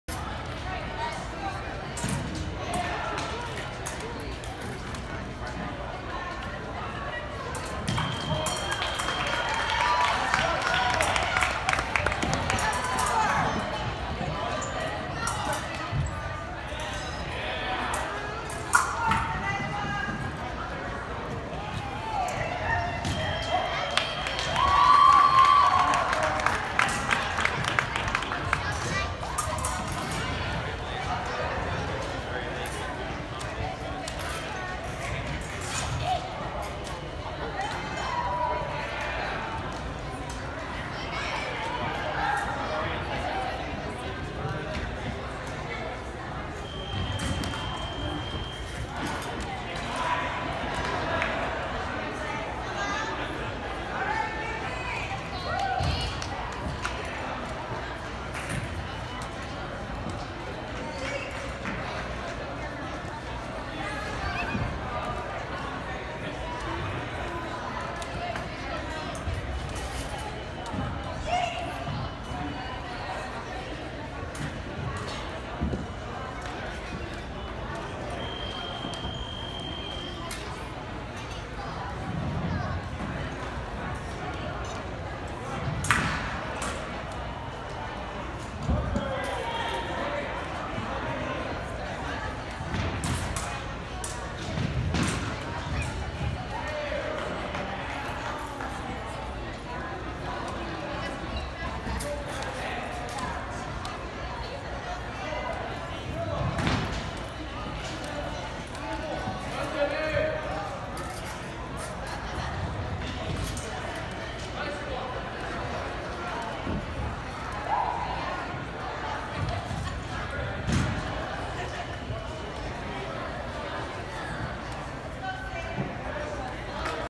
Hazel fencing
Hazel is facing the camera. Hazel won 5-1